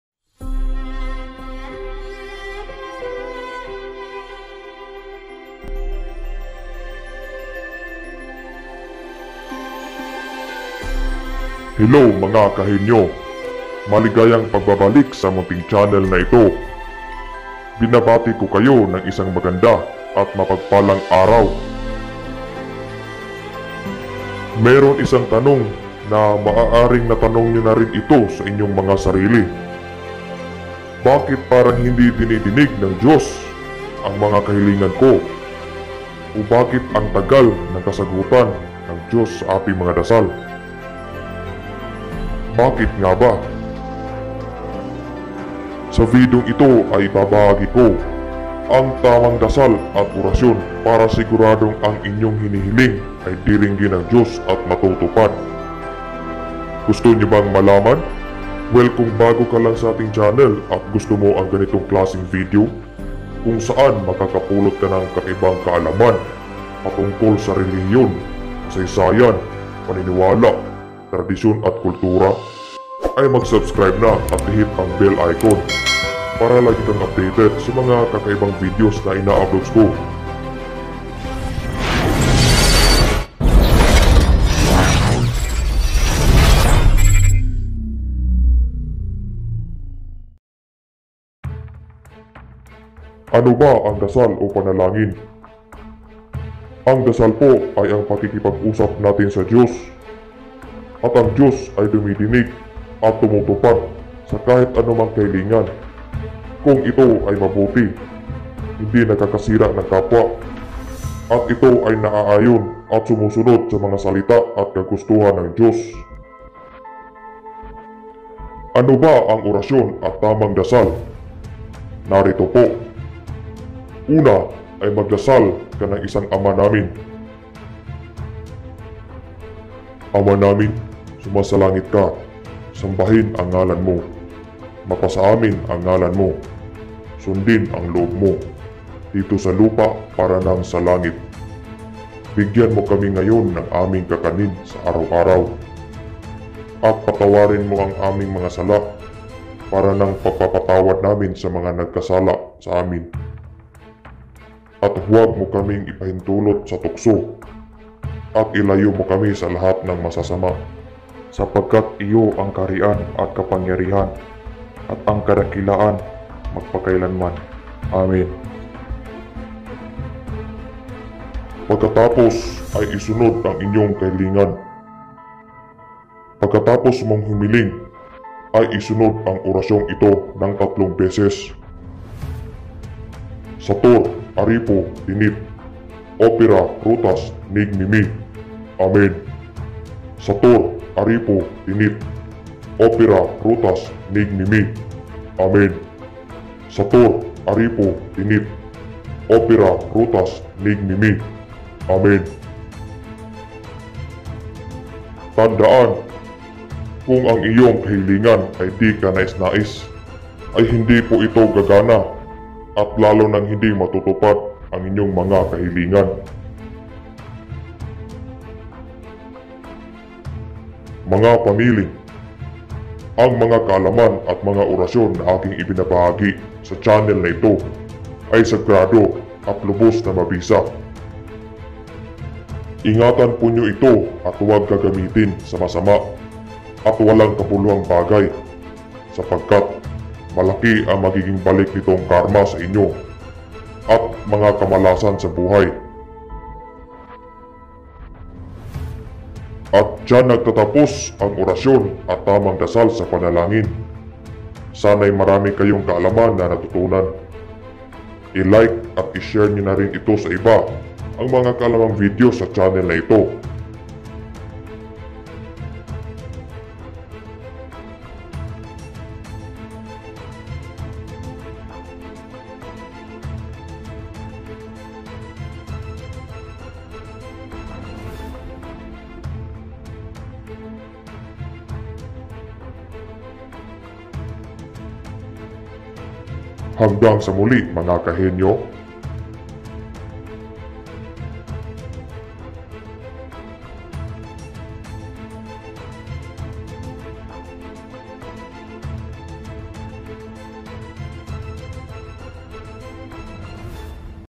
ORACION AT TAMANG DASAL SA PANALANGIN | SIGURADONG MATUTUPAD | MSPH
Hello mga ka Henyo!!

Ang oracion pong ito sa panalagin ay mabisa talaga upang matutupad ang mga panalangin.
Pero dapat ang panalangin po ay naaayon sa mga salita ng Diyos na nasa bible.

All videos and images are credit to Google.com & Pixabay.com

Content idea:
Youtube.com/Karunungan ng Diyos

SUPPORTAHAN PO NATIN ANG MGA SMALL YOUTUBER NA GAYA KO.

Salamat at God bless us all,
By: MindServer PH